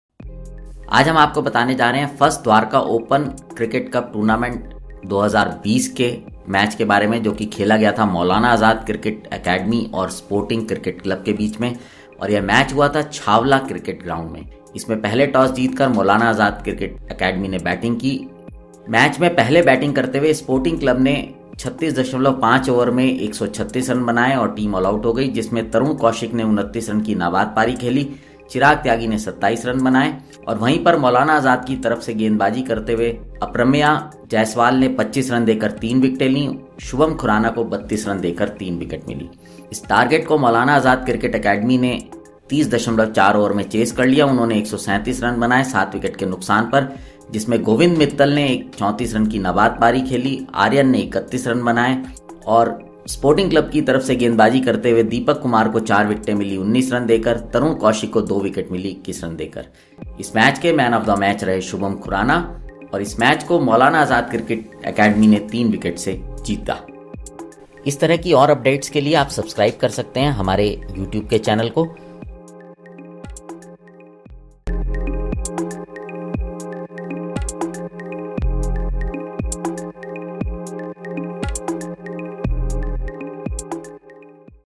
Maulana Azad wins inaugural match of First Dwarka Open Cricket Cup
Match Summery

 MAULANA AZAD CRICKET ACADEMY V/S SPORTING  CRICKET CLUB

Toss Won By Maulana Azad Cricket Academy & decided to bowl first

SPORTING CRICKET CLUB :~ 136/10,36.5 Overs.

Tarun Kaushik.         :~ 29* Runs
Chirag Tyagi.             :~ 27 Runs

Aprameya Jaiswal.  :~ 25/3 (7.5)
Shubham Khurana.  :~ 32/3 (8)

MAULANA AZAD CRICKET ACADEMY  :~ 137/7,30.4, Overs.

Govind Mittal.           :~ 34* Runs
Aryan..                        :~  31 Runs

Deepak Kumar.          :~ 19/4 (7)
Tarun Kaushik.           :~  21/2 (7)

M.O.M.     :~ SHUBHAM KHURANA.  32/3 (8)

MAULANA AZAD CRICKET ACADEMY  Won By 3 WICKETS.